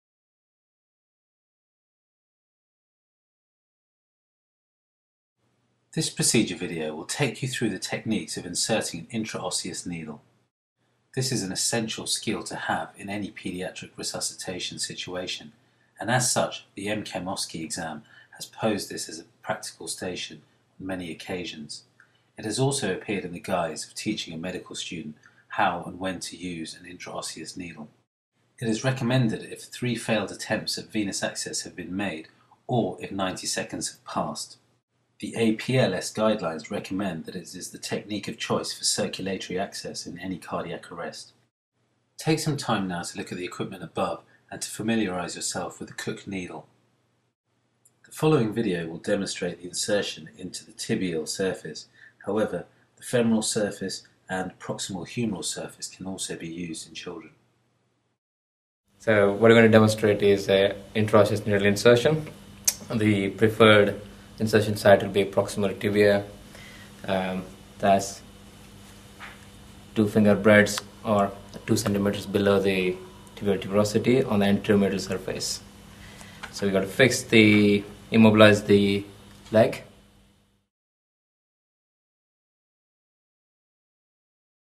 Intraosseous Needle Insertion procedure by Bromley Emergency Courses
An intraosseous needle insertion procedure video which highlights techniques examined in both the MRCEM & FRCEM exam.